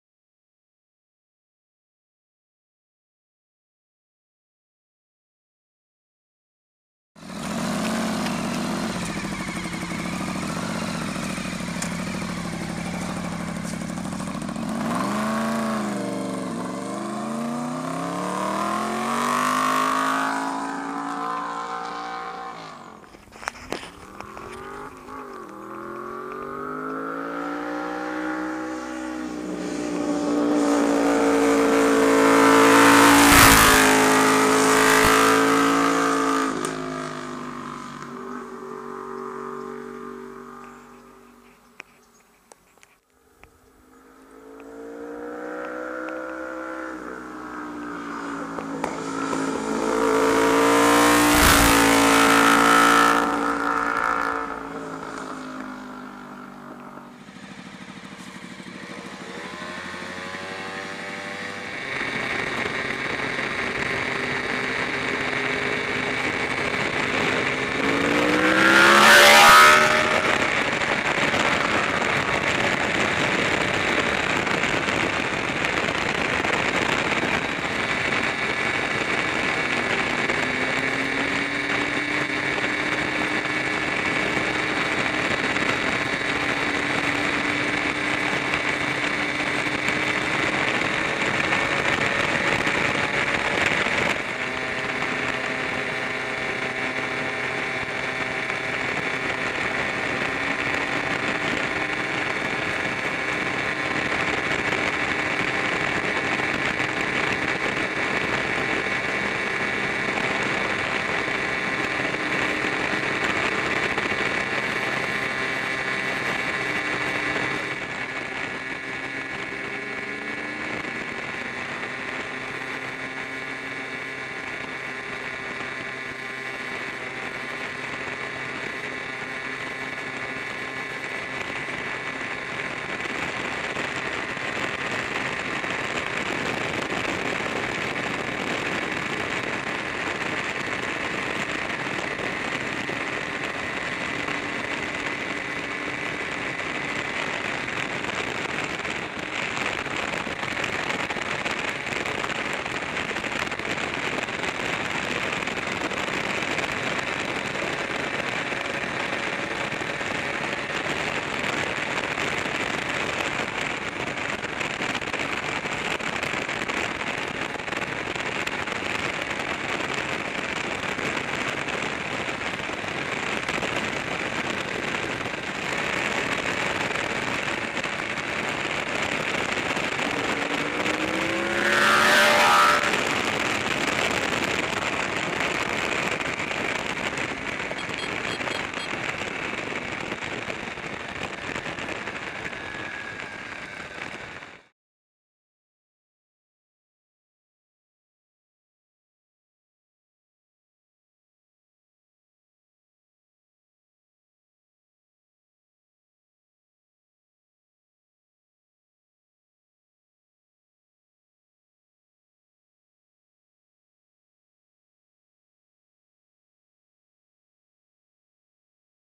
Having Fun
Doing a couple fly bys and mark pulling the chase cam on his scooooooooter!